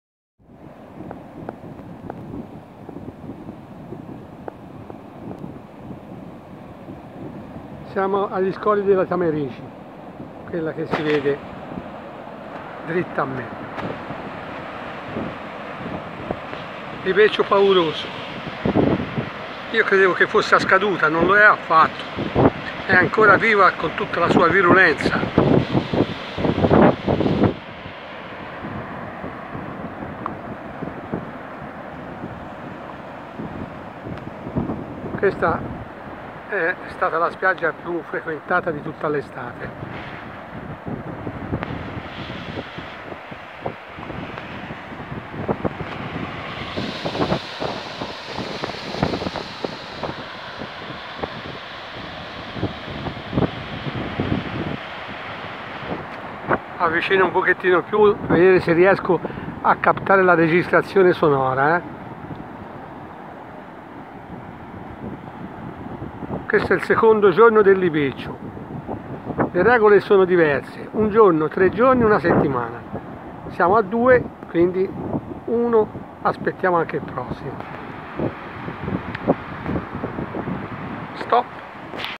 **LIBECCIATA allo scoglio della BALLERINA !!
un meraviglioso mare di verde smeraldo questa grande e vasta scogliera  ,in estate,è frequentata da centinaia di livornesi a fare il bagno con cestini di cibo e vettovaglie !!